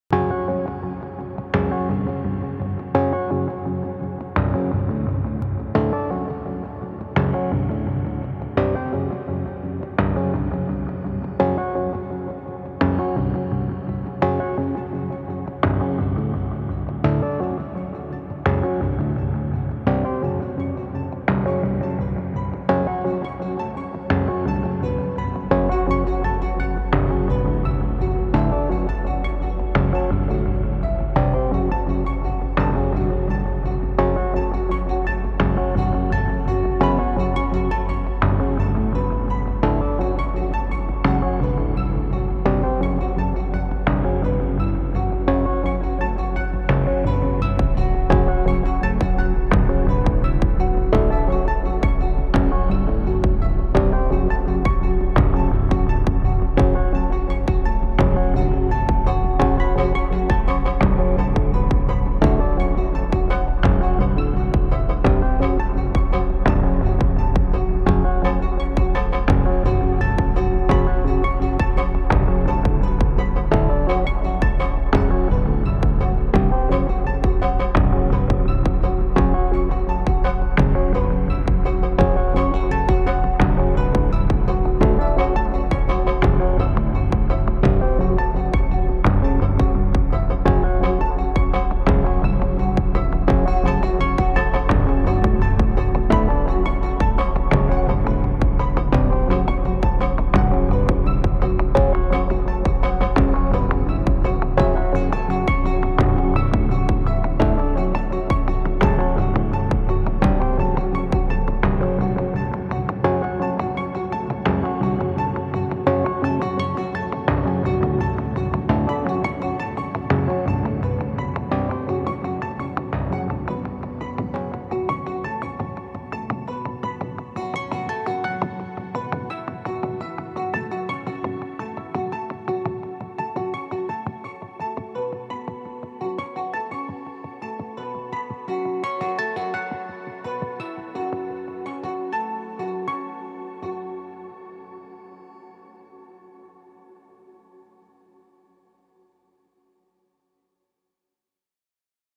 Make it quick - A eurorack synthesiser patch
I like to patch the modular every Saturday. I make an effort to use saturday productively. I'm lucky to have that time.  Today's patch is for the time poor.